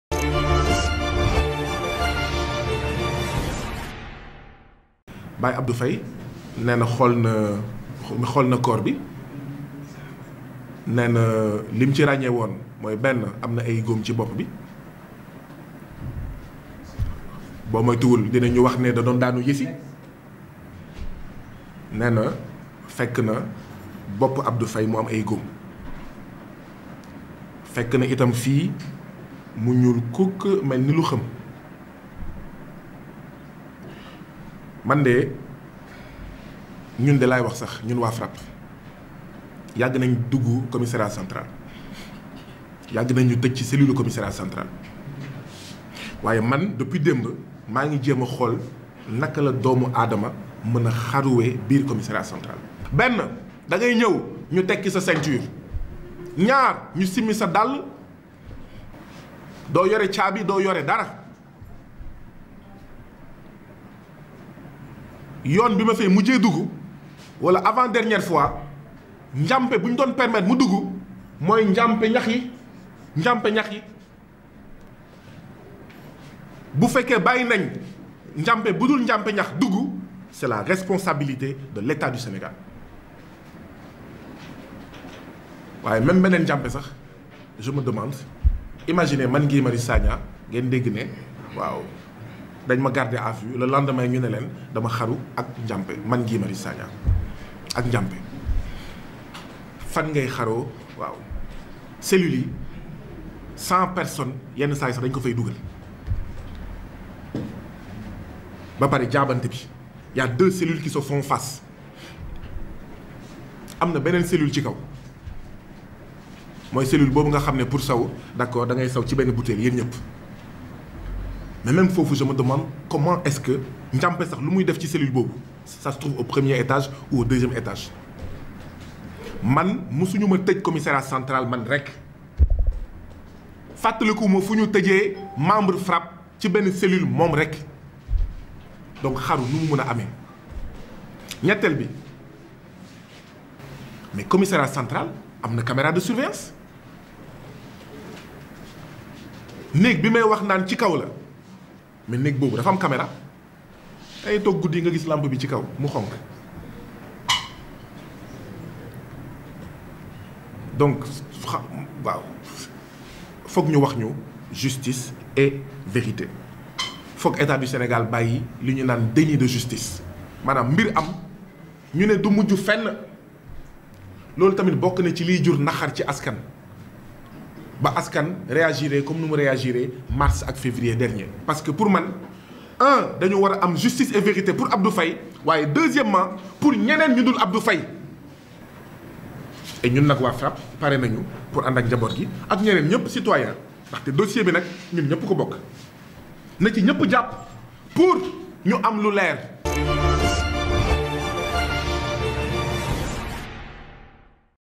Mort du détenu Abdou Faye: Guy Marius Sagna réfute la thèse du suicide et explique.
Suivez Guy Marius Sagna